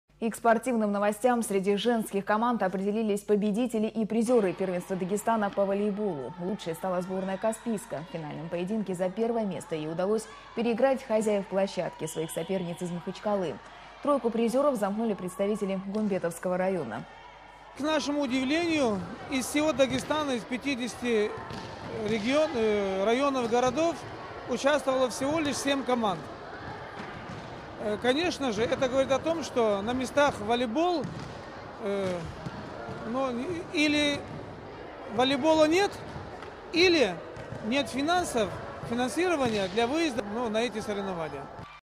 Определились победители первенства Дагестана по волейболу среди женских команд
Лучшей стала сборная Каспийска. В финальном поединке за первое место ей удалось переиграть хозяев площадки, своих соперниц из Махачкалы.